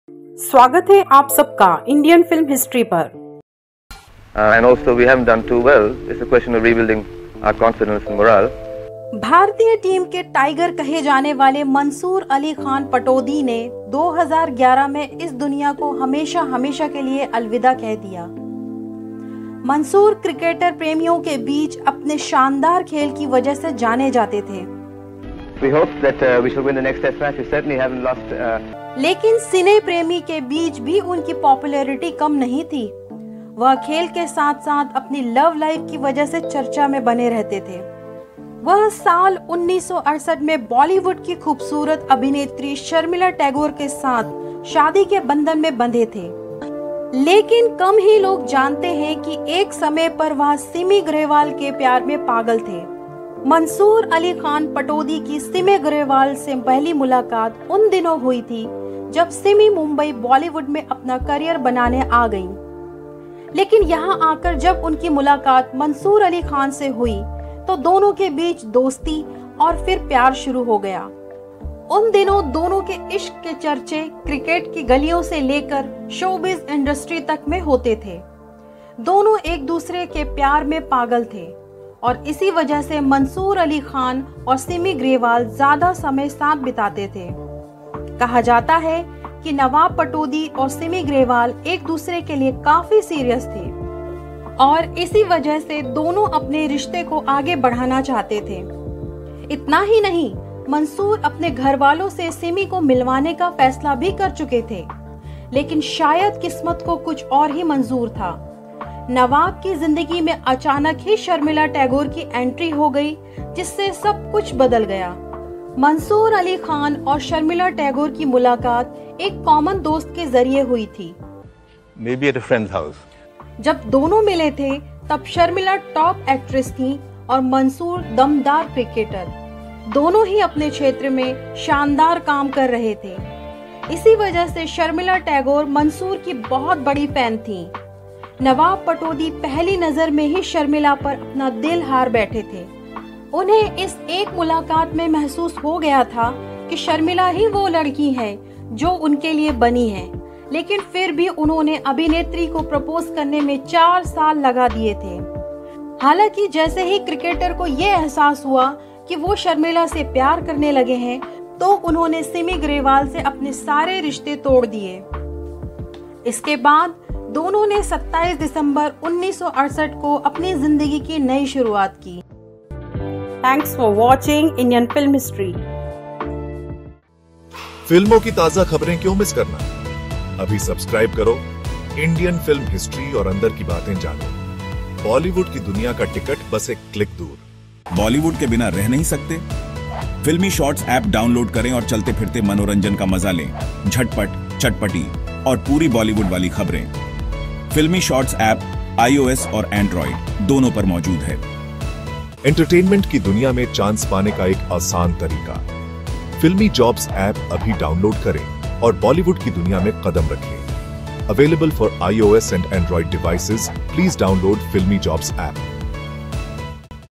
शर्मिला टैगोर के लिए मंसूर अली ने तोड़ा था इस एक्ट्रेस का दिल
Developed and Managed By: HUVI DIGI LIFE PRIVATE LIMITED